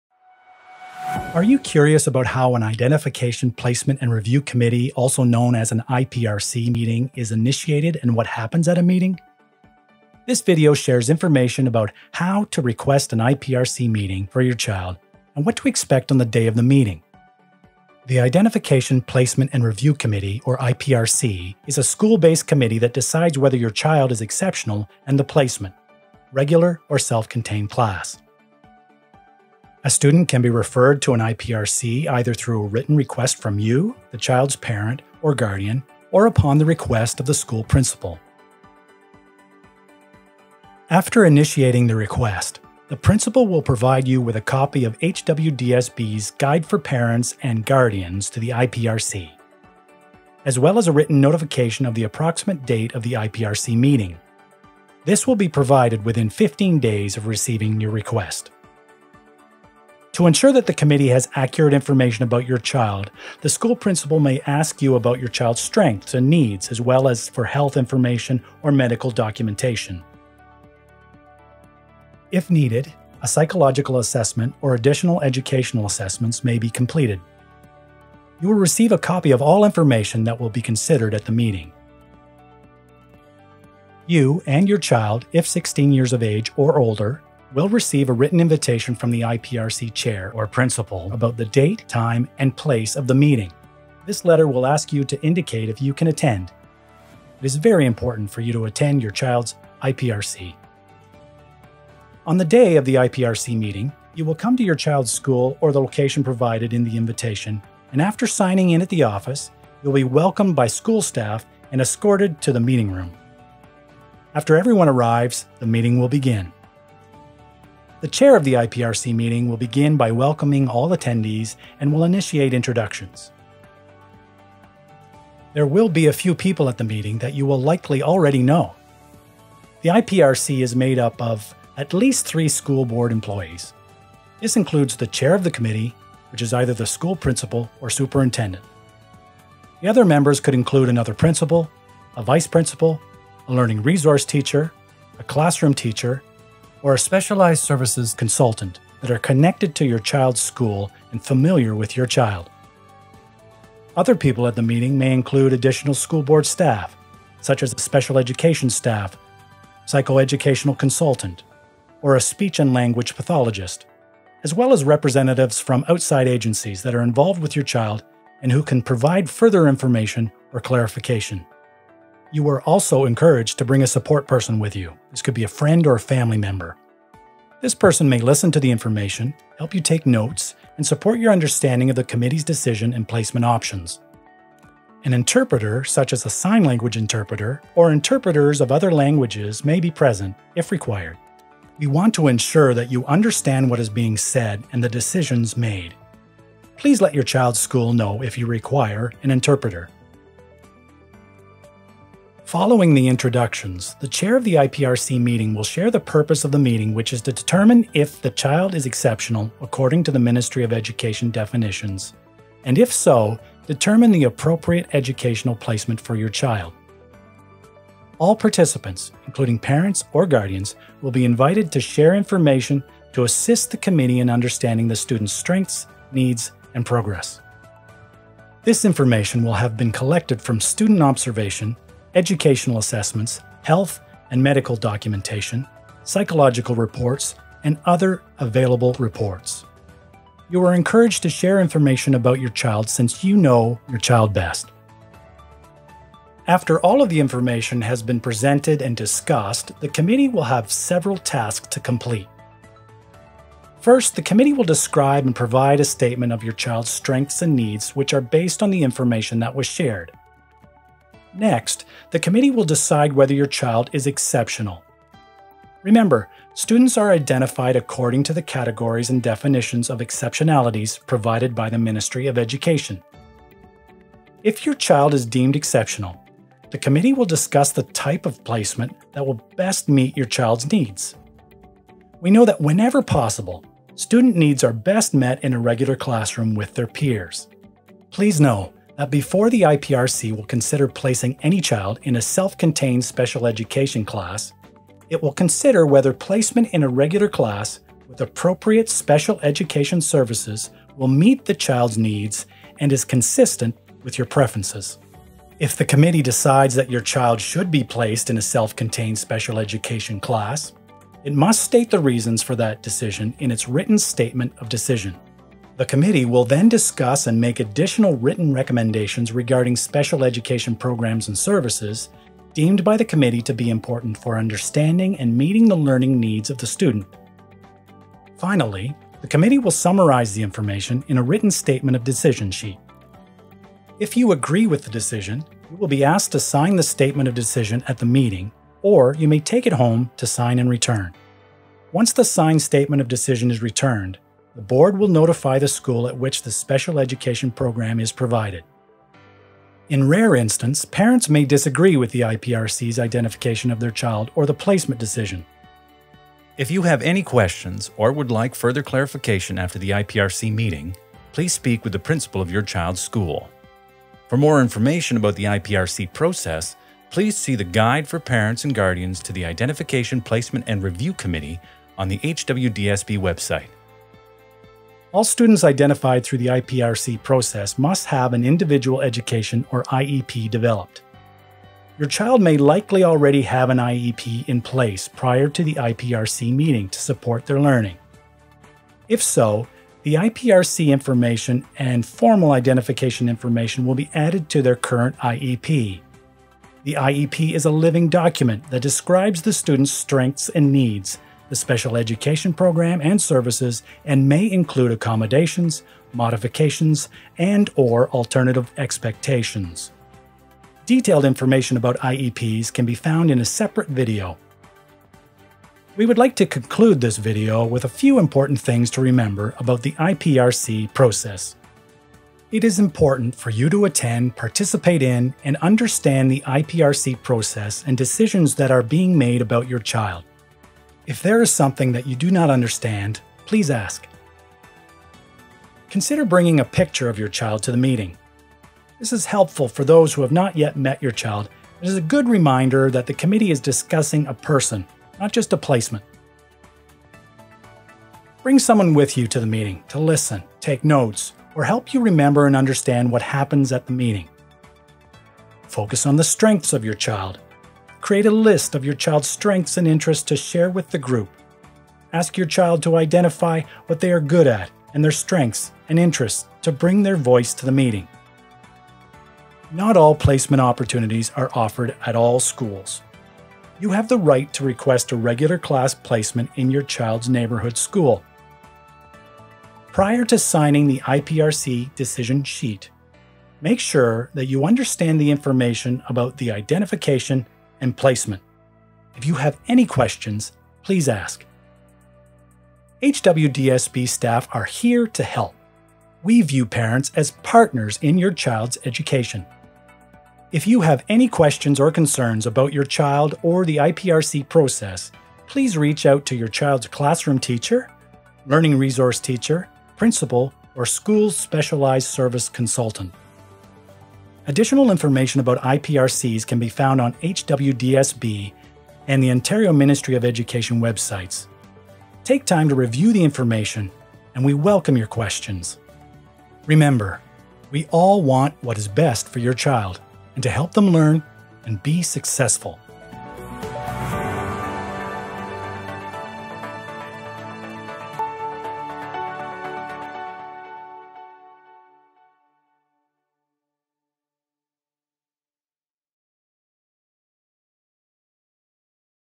IPRC Meetings | HWDSB
Produced and Voiced by LW MacVicar of VICARIUS
for
Specialized Services, Hamilton-Wentworth District School Board (HWDSB)

Learn more about what happens at meetings of the Identification, Placement and Review Committee in this video resource for parents, guardians and caregivers at HWDSB.

The Identification, Placement and Review Committee (IPRC) is a school board committee that decides whether your child is exceptional and the type of placement that will build on your child's strengths. Ministry of Education Regulation 181/98 governs the IPRC process.